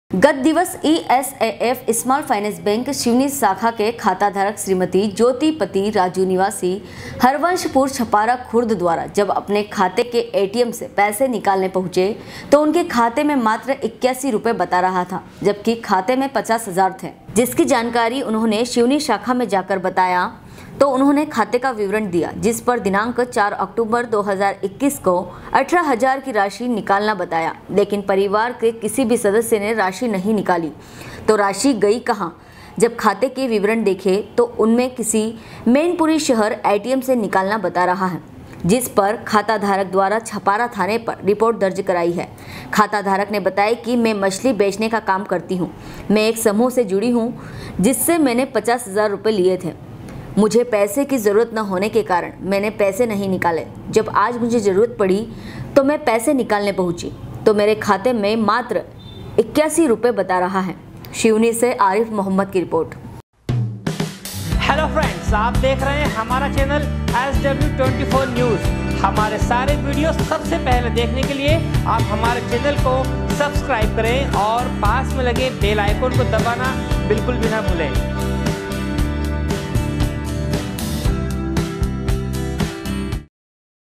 SW 24 News Madhyapradesh
गत दिवस ईएसएएफ स्माल फाइनेंस बैंक सिवनी शाखा के खाताधारक श्रीमती ज्योति पति राजू निवासी हरवंशपुर छपारा खुर्द द्वारा जब अपने खाते के एटीएम पैसे निकालने पहुंचे तो उनके खाते में मात्र 81 रुपये बता रहा था जबकी खाते में 50 हजार थें। सिवनी से आरिफ मोहम्मद की रिपोर्ट